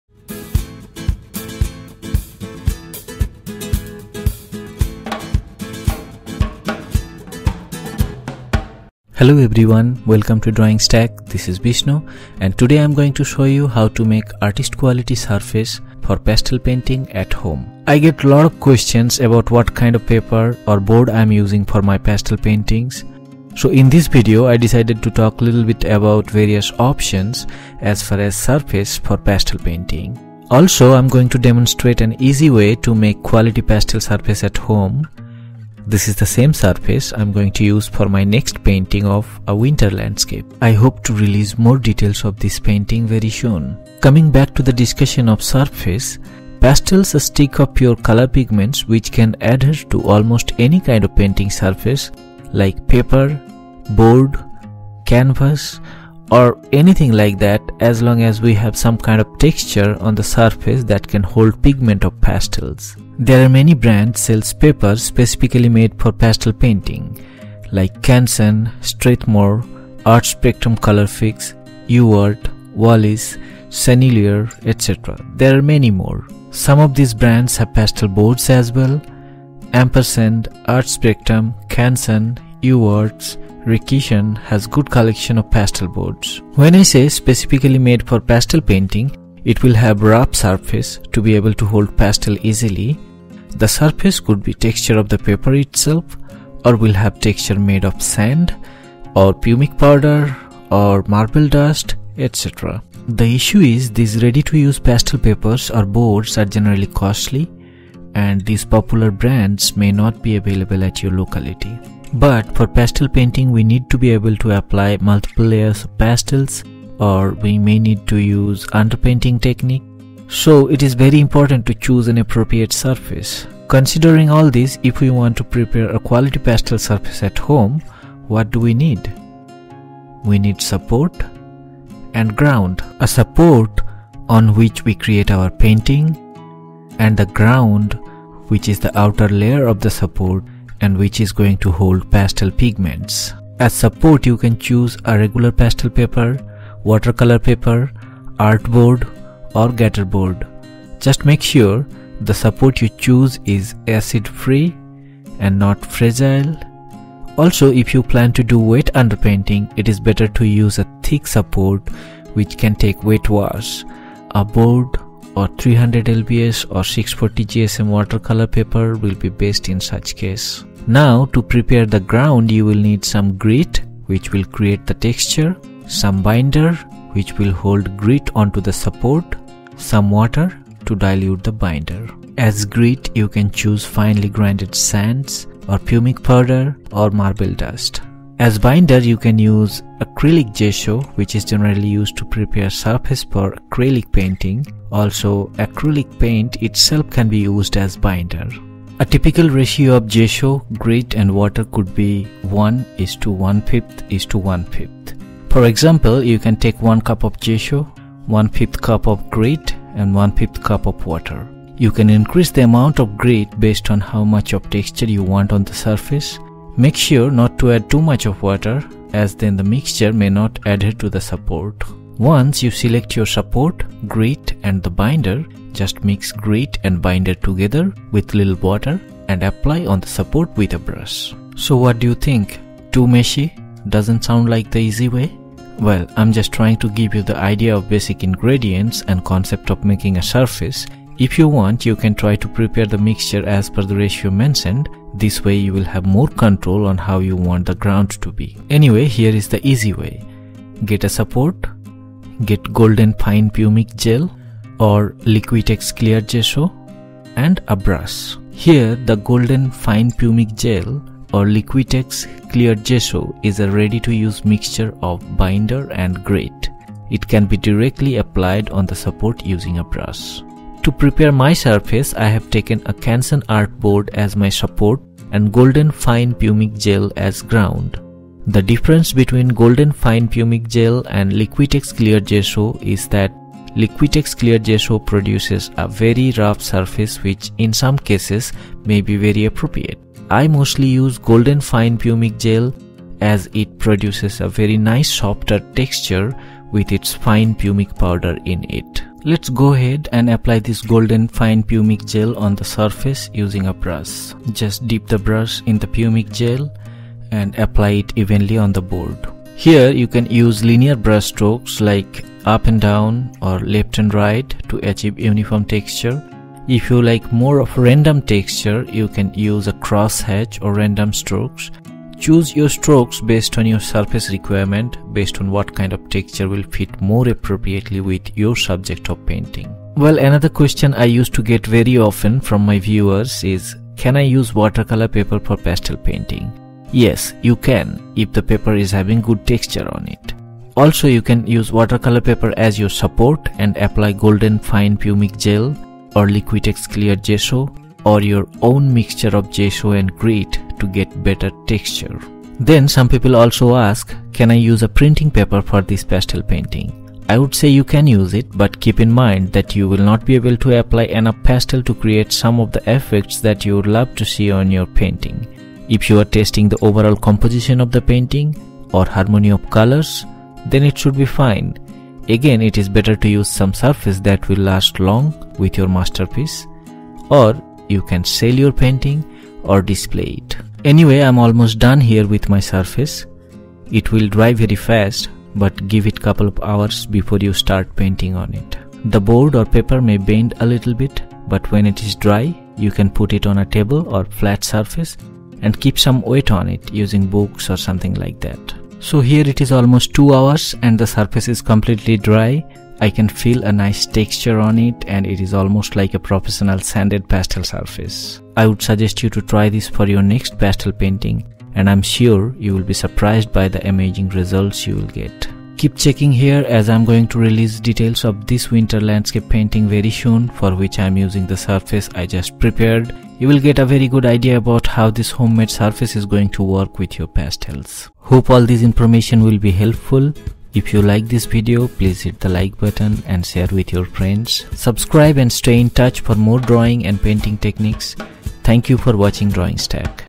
How to make artist quality surface for pastel painting at home - Homemade pastel surface
This is a video to explain details of surface options for pastel painting and to show how to make artist quality surface for pastel painting at home.

Various popular brands of pastel papers and boards are discussed in this video; which are -
Canson, Strathmore, Art Spectrum Colorfix, Uart, Wallis, Sennelier, Ampersand, Richeson, etc.

Also you will get details of various ingredients and formula for homemade pastel surface. At the end there is a demonstration to show one quick and easy way to make pastel surface using Golden Fine Pumice Gel or Liquitex Clear Gesso.

Let's watch the video for more details and let me know if you like this. Thanks you.